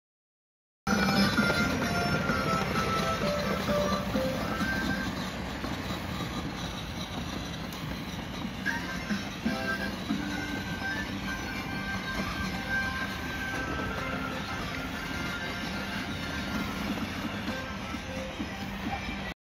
Malik drive his mclaren
مالك يقود سيارته ماكلارن في الحديقه malik driving his mclaren at the park